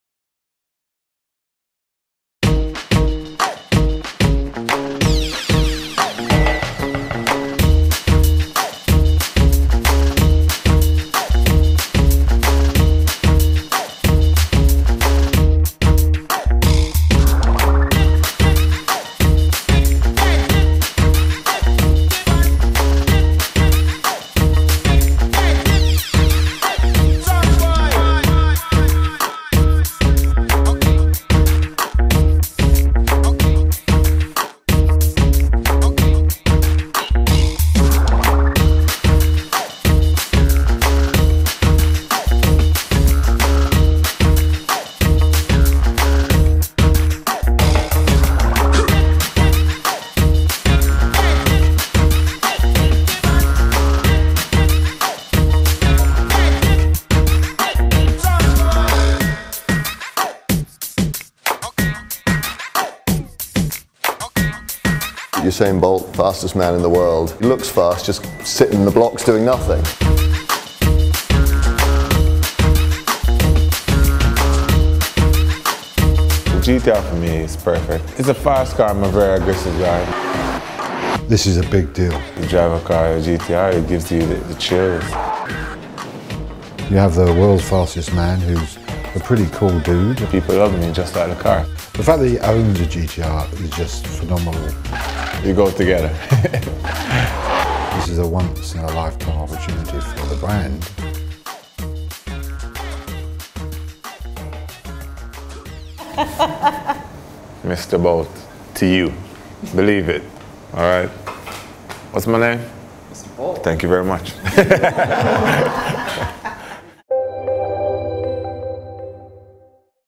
Nissan Behind-the-Scenes with Usain Bolt
Nissan partners with Usain Bolt, Olympic and world record holder and world's fastest man, to launch a new global brand campaign. Here's the behind-the-scenes look at the making of the multimedia creatives in Usain's hometown of Kingston, Jamaica.